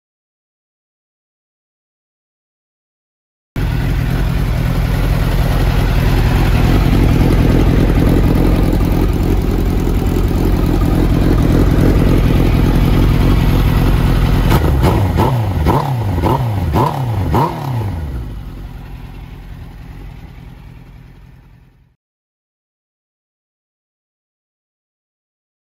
SUZUKI GSX-R1000 × YOSHIMURA Titanium Full Exhaust Muffler／ヨシムラ製トライオーバルサイクロン ツーエンドタイプを装備（3655）
京都のバイクショップ スペックMです。
カスタムの参考に、マフラーの音と装着イメージをご覧ください。